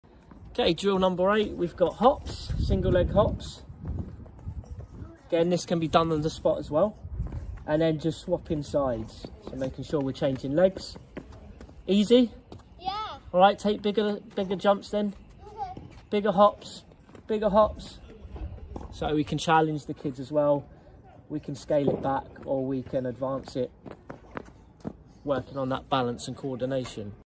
Tennis Drill 8 - Warm Up - Single Leg Hops
Simple tennis drills and skills for kids. The aim is to share content for all abilities and without the need for a tennis court or even a racket. Teaching fundamental athletic tennis movements that will improve your game!